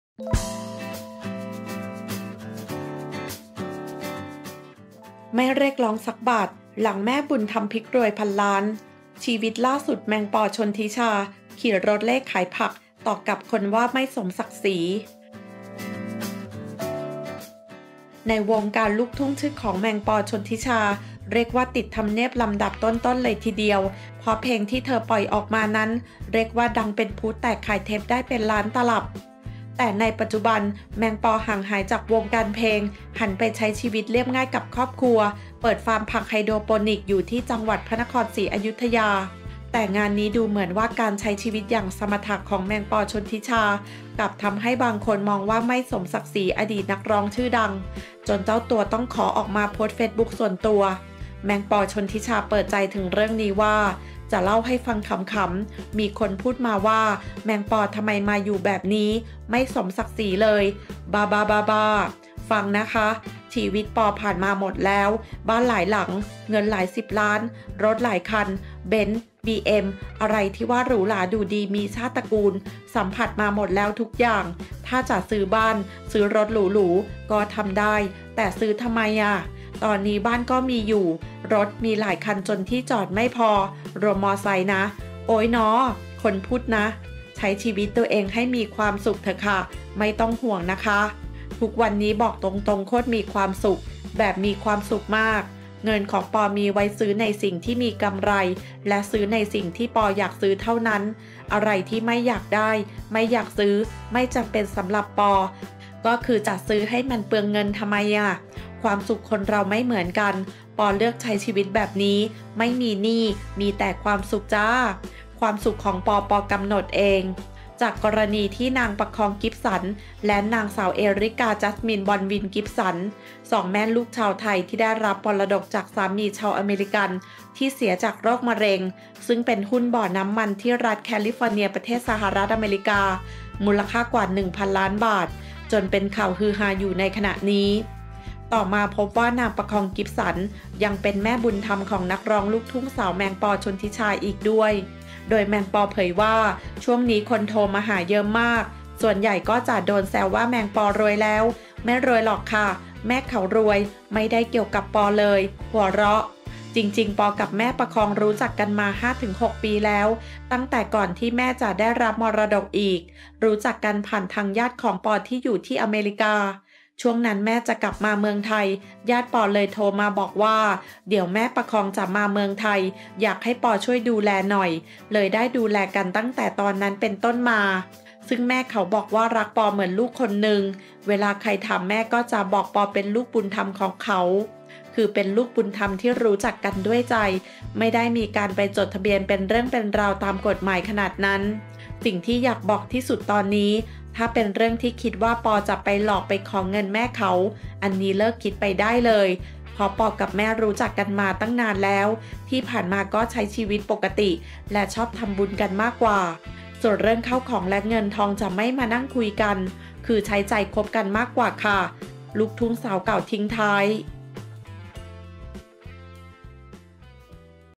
ไม่เรียกร้องซักบาท! หลังแม่บุญธรรมพลิกรวยพันล้าน ‘แมงปอ ชลธิชา’ ขี่รถเร่ขายผัก ตอกกลับคนว่า
ไม่เรียกร้องซักบาท! หลังแม่บุญธรรมพลิกรวยพันล้าน ชีวิตล่าสุด ‘แมงปอ ชลธิชา’ ขี่รถเร่ขายผัก ตอกกลับคนว่าไม่สมศักดิ์ศรี

ในวงการลูกทุ่งชื่อของ แมงปอ ชลธิชา เรียกว่าติดทำเนียบลำดับต้นๆ เลยทีเดียว เพราะเพลงที่เธอปล่อยออกมานั้นเรียกว่าดังเป็นพลุแตก ขๅยเทปได้เป็นล้านตลับ